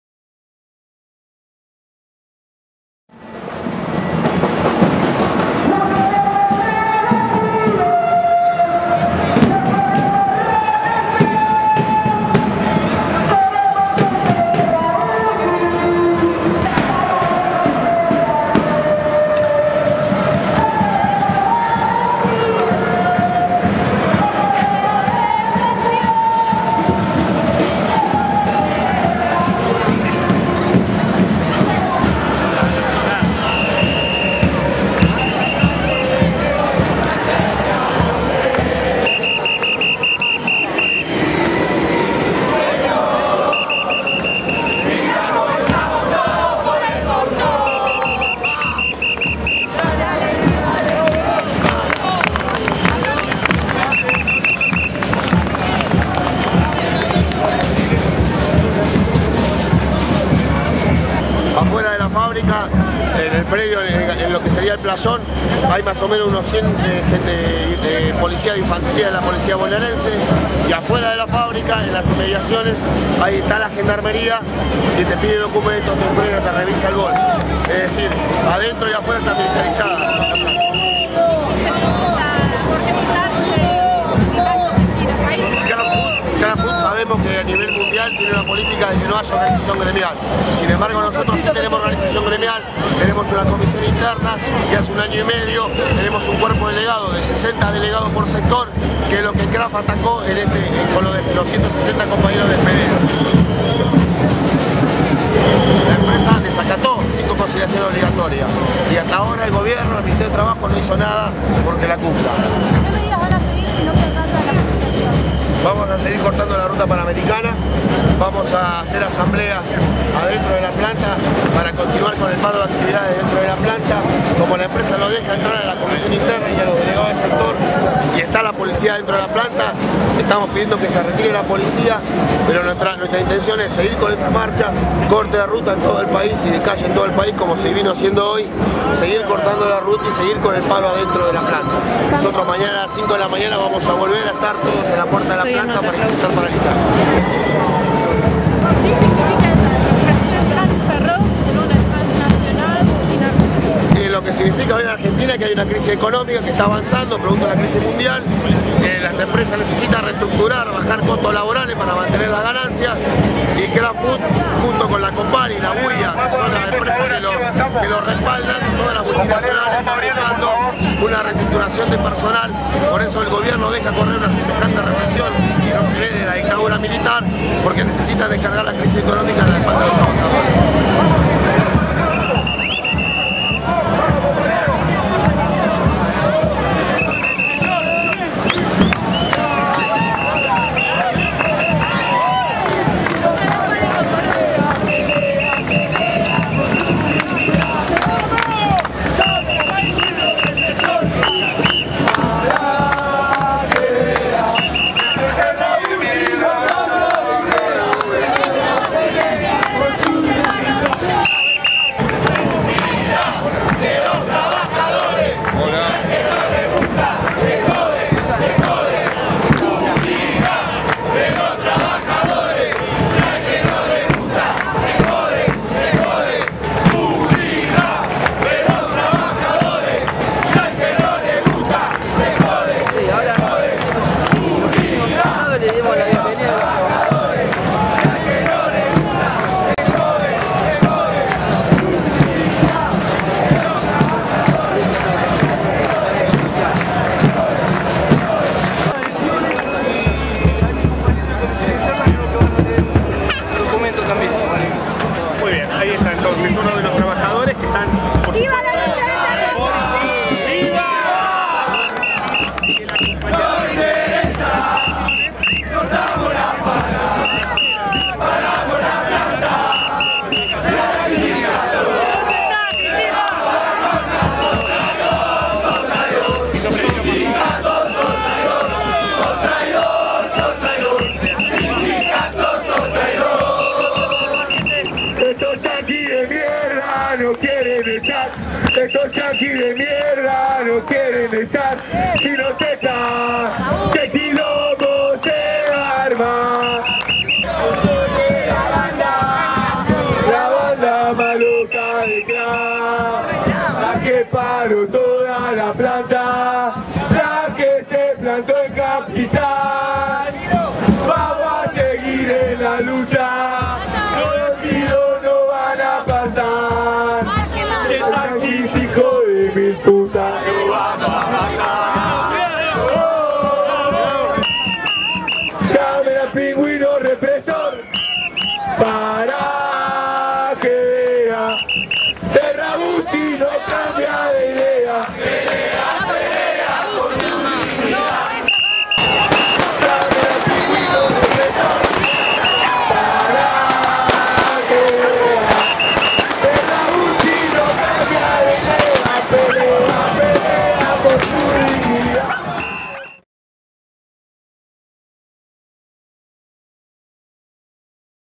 Marcha contra la represión a los trabajadores de Kraft - Terrabusi 28 09 09
Movilización de Congreso a Plaza de Mayo en solidaridad y apoyo con los trabajadores despedidos y contra el desalojo y la represión del pasado viernes en la fábrica Kraft - Terrabusi en Pacheco.